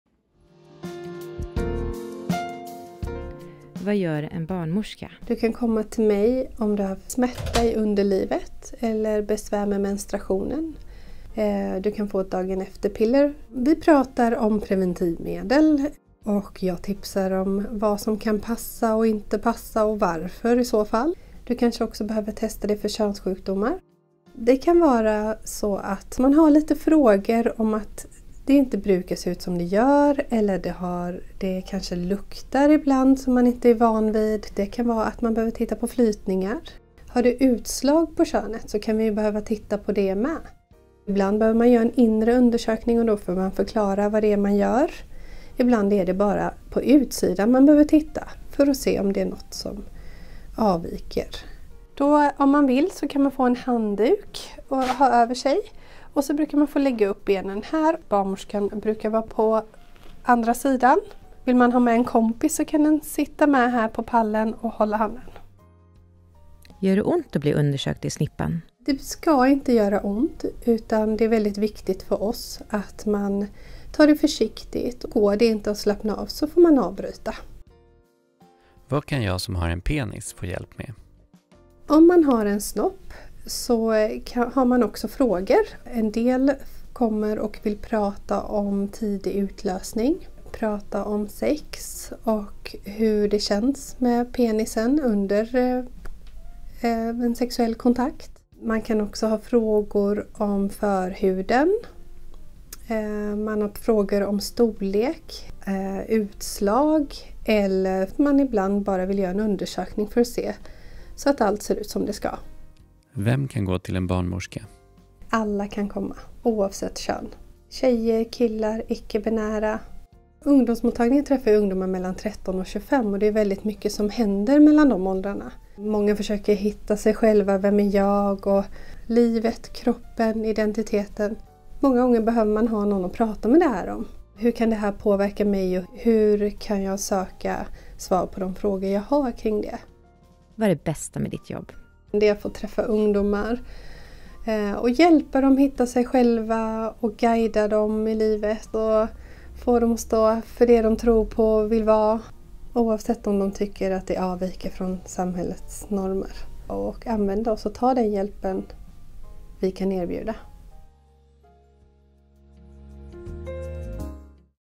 Vad gör en barnmorska?
Eivan Heidari jobbar som barnmorska på en ungdomsmottagning. Här berättar hon om sitt jobb.